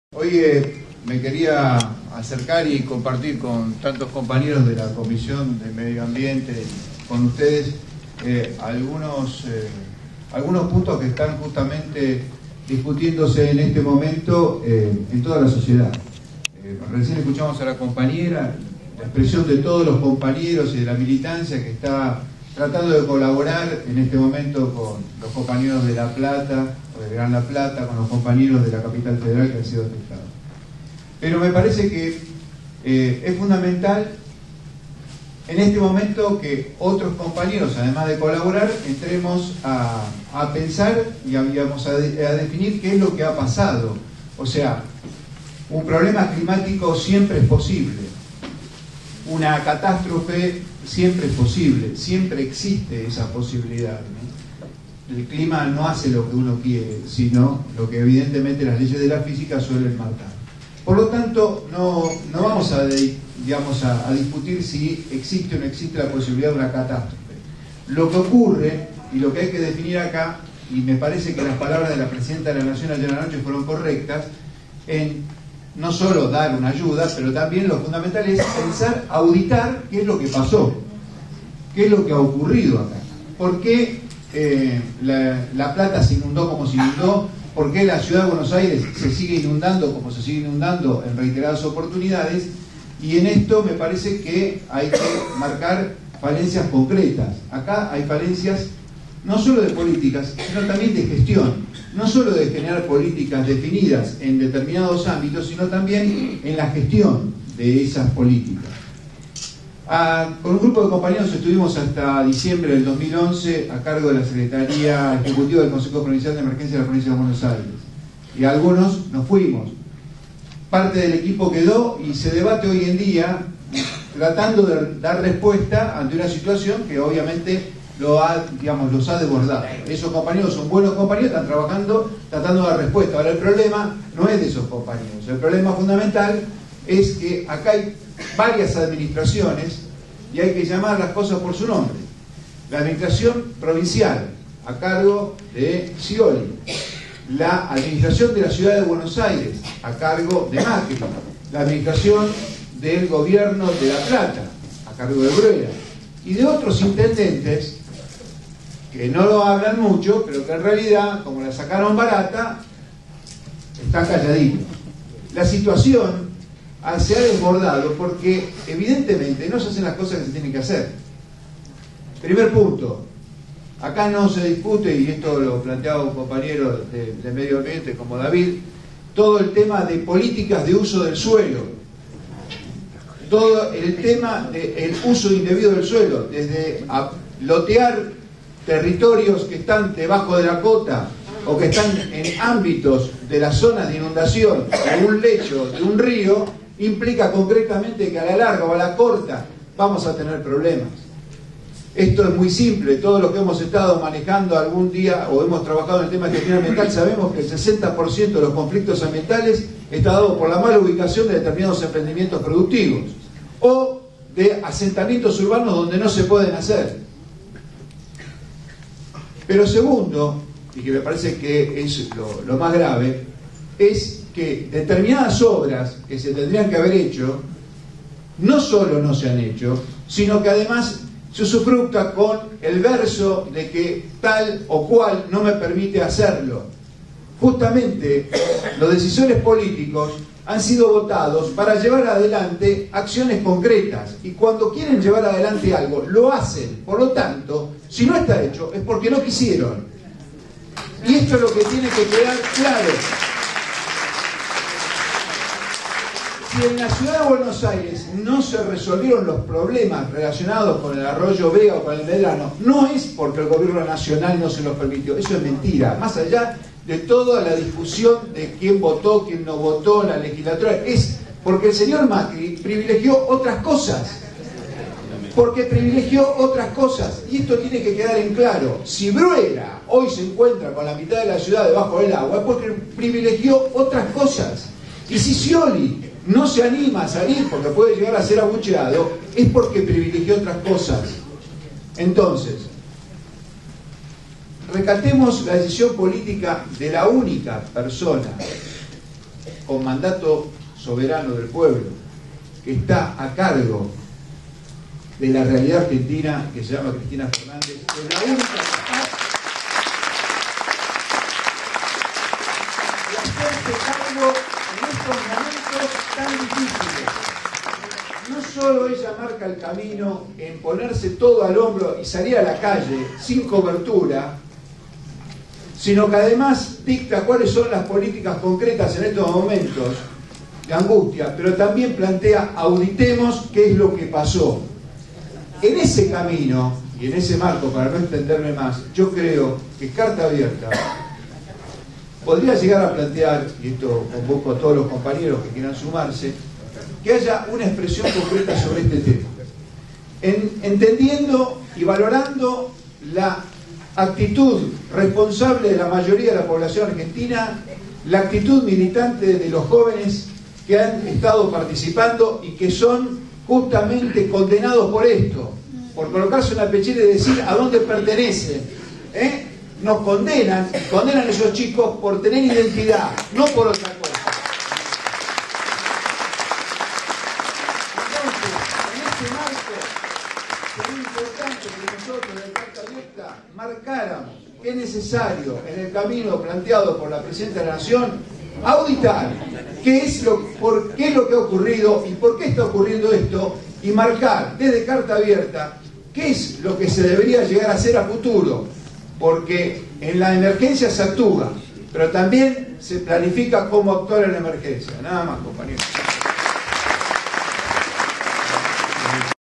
03CA_ Jorge Etcharran_Emergencias
Asamblea de Carta Abierta, sábado 6 de abril de 2013.Biblioteca Nacional.Buenos Aires